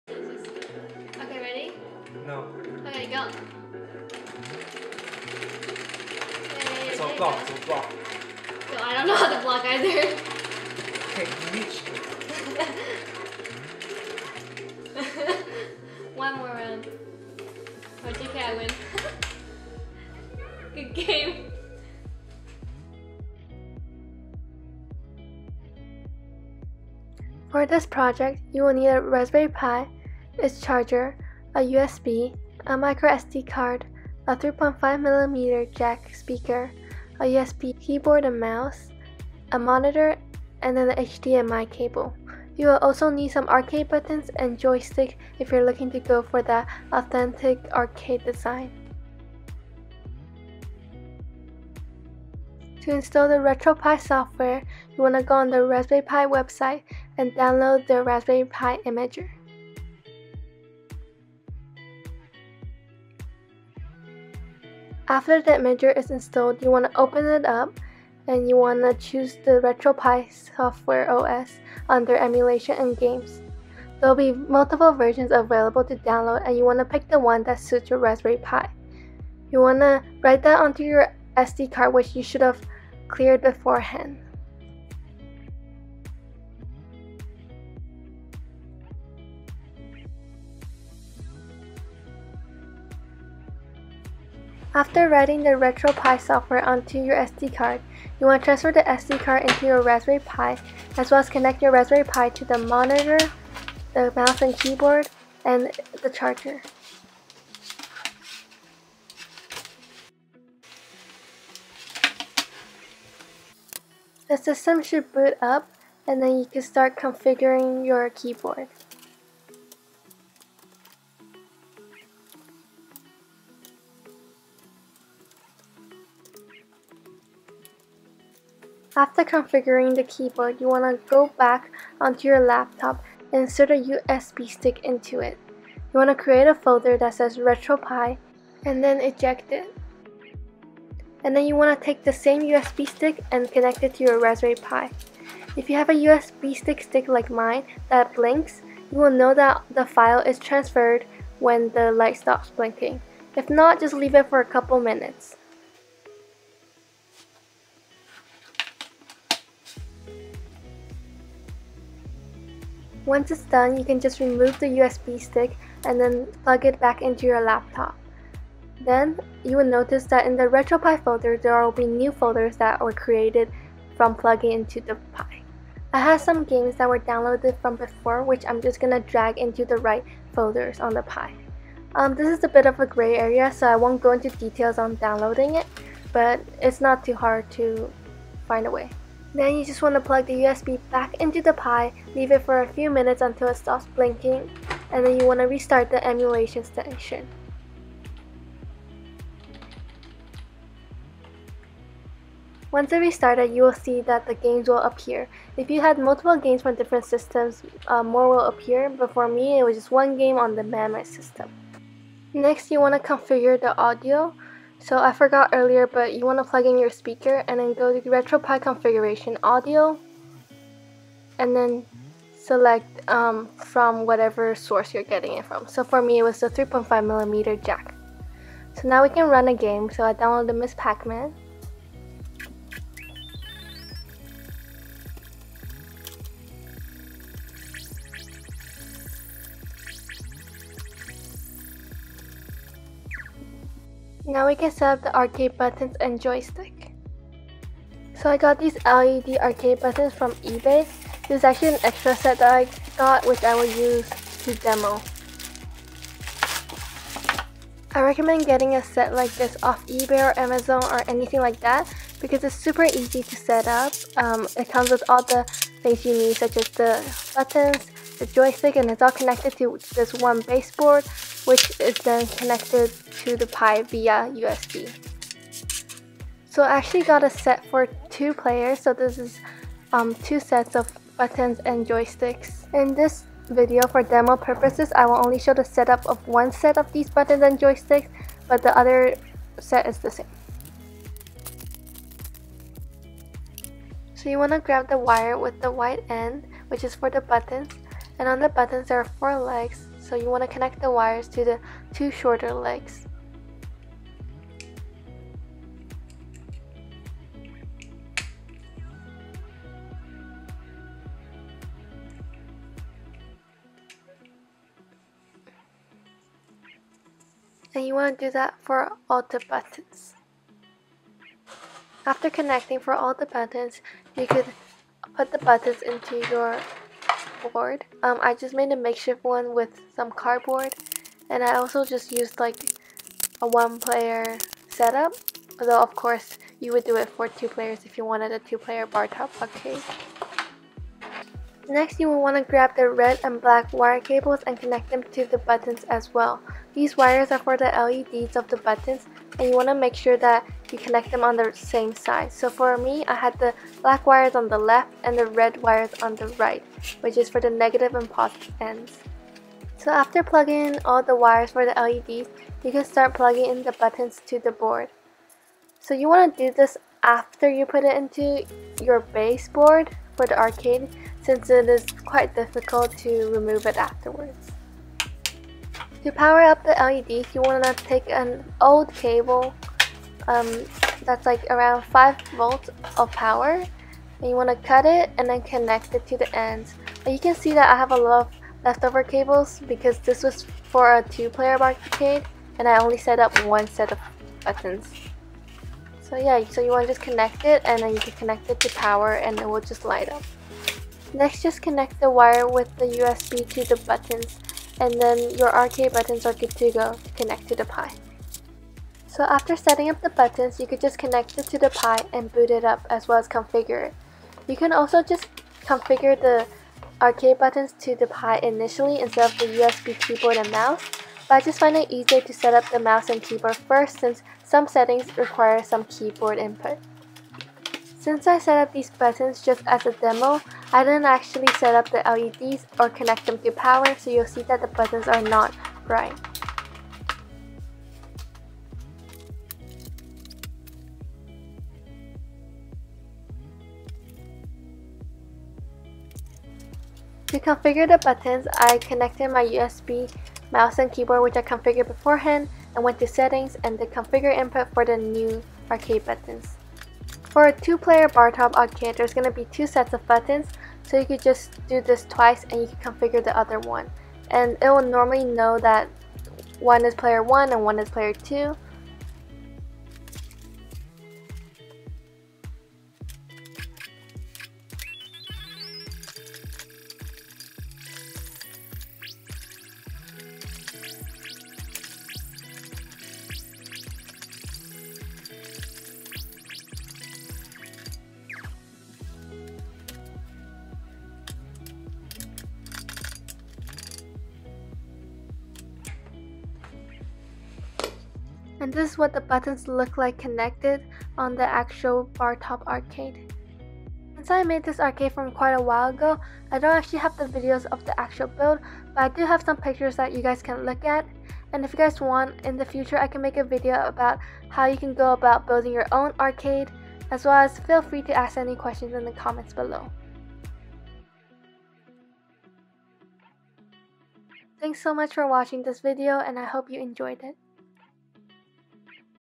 RETROPIE BARTOP ARACADE | COMPLETE SOFTWARE AND BUTTONS SETUP TUTORIAL
Hi guys!

In this video, I will show you how to set up the software of a raspberry pi powered 2-player bartop arcade (updated way 2021). I will also show you how to set up the arcade buttons + joysticks.

Since I made this arcade a while ago, I don't have much footage of the actual build itself. However, if you guys are interested I could make a detailed video on how I planned and ultimately build my arcade cabinet or just a short video showcasing my arcade.

I hope you guys enjoy and feel free to use the timestamps down below!

0:00 Intro
0:24 What you need
0:51 Installing the Retropie Software
1:31 Setting up the Arcade
4:04 Setting up the Arcade Buttons + Joysticks
8:18 Pictures of the Actual Build